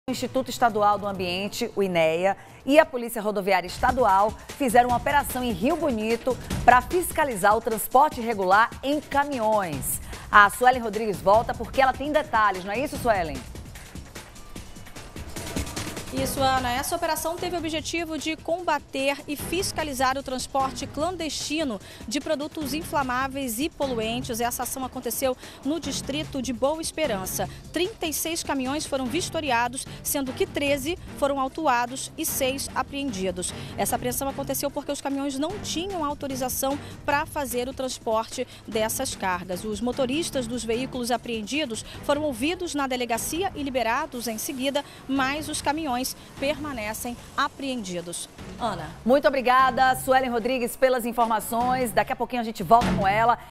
Em Rio Bonito, INEA e BPRV combatem o transporte de produtos em caminhões
O Instituto Estadual do Ambiente (INEA) e a Polícia Rodoviária Estadual fizeram uma operação em Rio Bonito para fiscalizar o transporte irregular em caminhões.